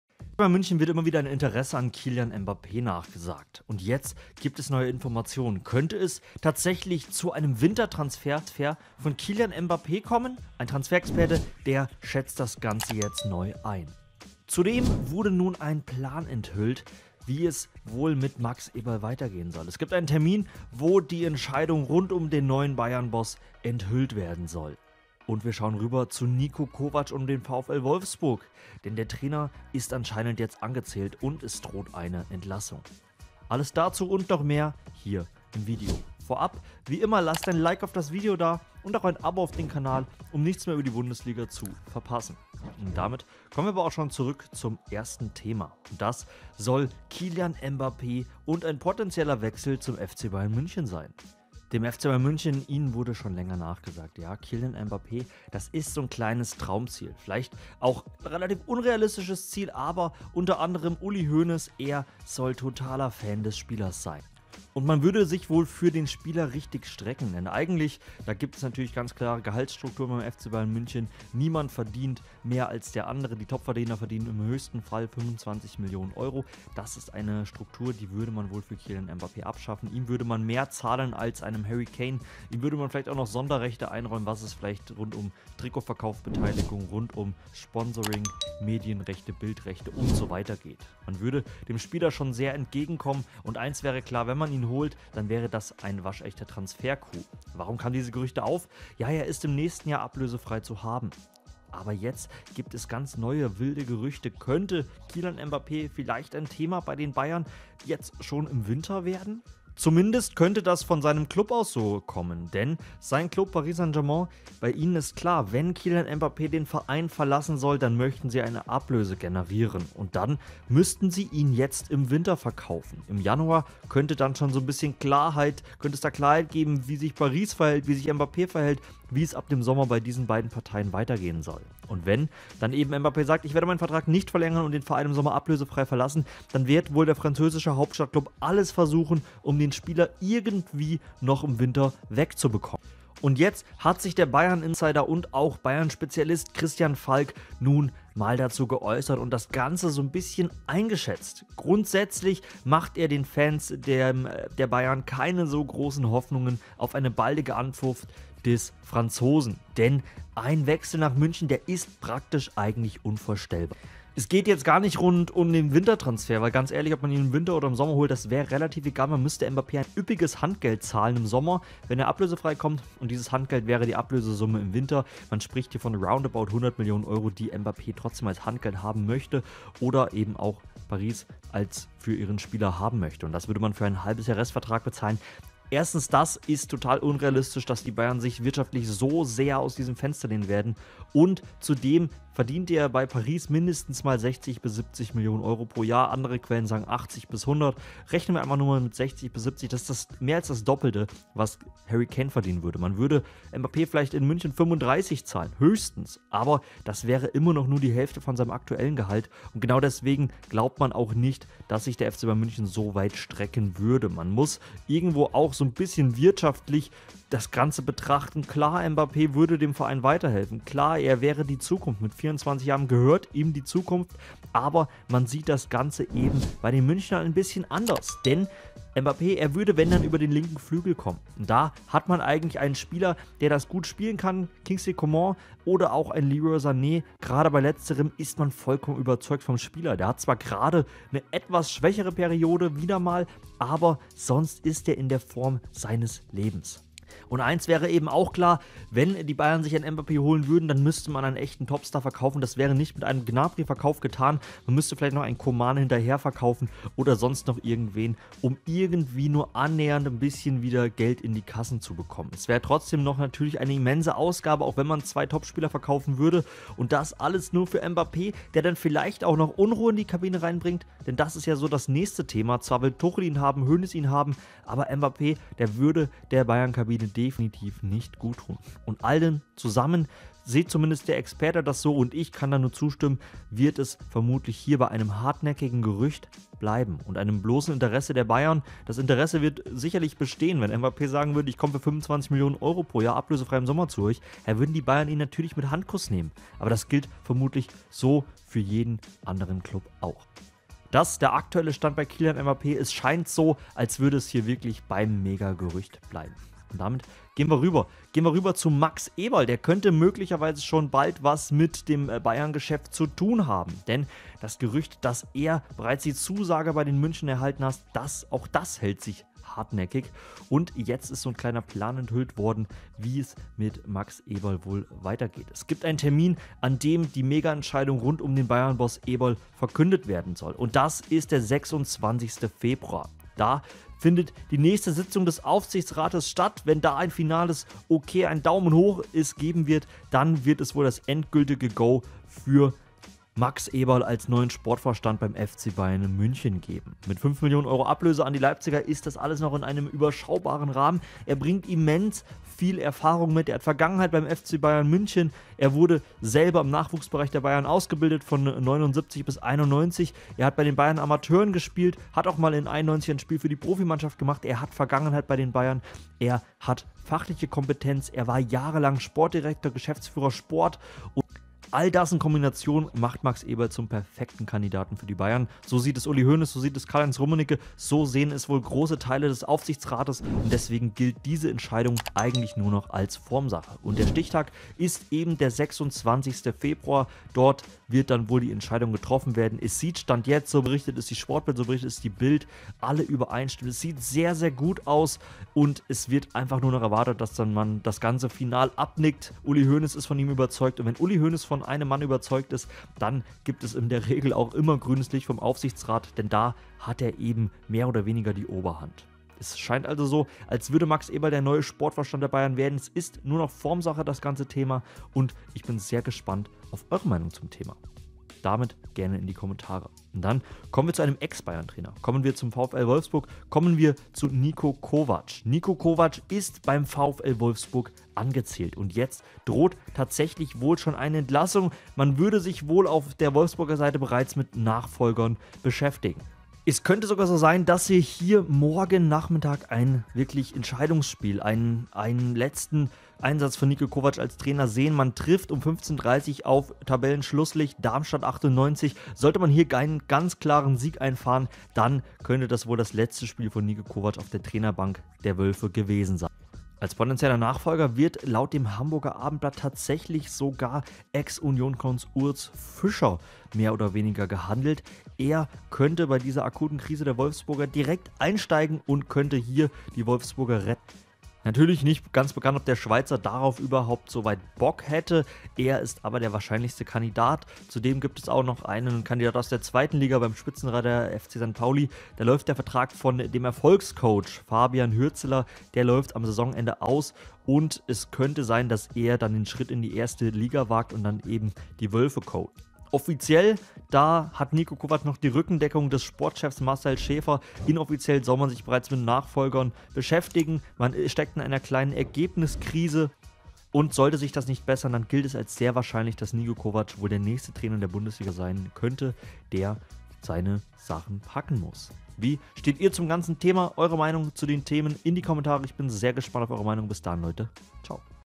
Kylian MBAPPE vor Winter-Transfer zu BAYERN?! Bayern-PLAN mit Max EBERL enthüllt!
Kommt es zum Transfercoup der Bayern bei Mbappe schon in diesem Winter? Und wann steht die Entscheidung bezüglich Max Eberl als neuer Bayern-Sportvorstand bevor? Zudem geht es um Niko Kovac und eine mögliche Entlassung! Alles dazu hier!